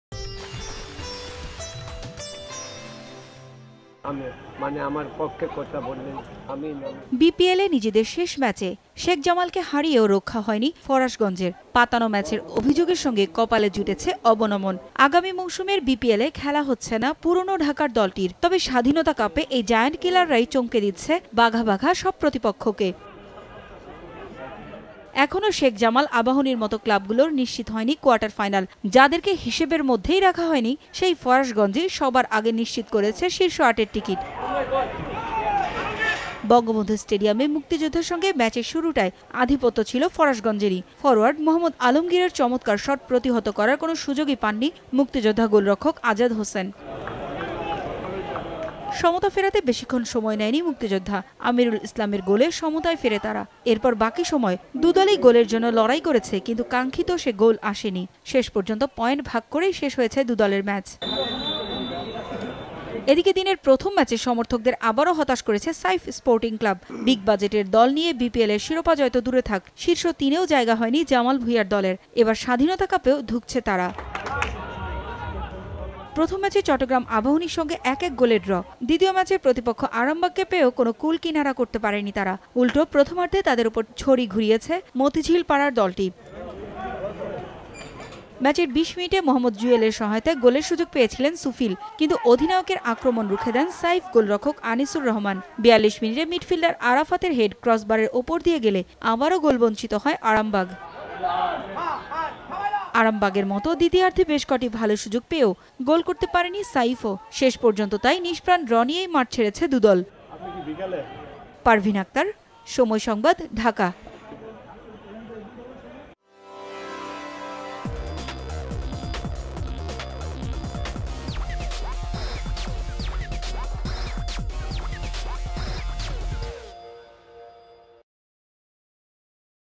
স্বাধীনতা কাপে সবার আগে কোয়ার্টার ফাইনালে ফরাশগঞ্জ ! | Independence Cup BD
বিপিএলে নিজেদের শেষ ম্যাচে শেখ জামালকেও হারিয়েও রক্ষা হয়নি ফরাশগঞ্জের। পাতানো ম্যাচের অভিযোগের সঙ্গে কপালে জুটেছে অবনমন। আগামী মৌসুমের বিপিএল খেলা হচ্ছেনা পুরোনো ঢাকার দলটির। তবে, স্বাধীনতা কাপে এই জায়ান্ট কিলাররাই চমকে দিচ্ছে বাঘা বাঘা সব প্রতিপক্ষকে।

এখনও শেখ জামাল, আবাহনীর মত ক্লাবগুলোর নিশ্চিত হয়নি কোয়ার্টার ফাইনাল। যাদেরকে হিসেবের মধ্যেই রাখা হয়নি, সেই ফরাশগঞ্জই সবার আগে নিশ্চিত করেছে শীর্ষ আটের টিকিট।

In the last match of the BPL, Sheikh Jamal was also not lost in the Farashganj. With the allegations of match-fixing in the forehead, The old Dhaka team will not be playing next BPL in the coming season. However, these giant killers in the Independence Cup will surprise all the opponents of Bagha Bagha.

There are still clubs like Sheikh Jamal, Abahani, and the quarter-finals are not sure. Those who were not kept in the reckoning, the Farashganj confirmed the first highest ticket.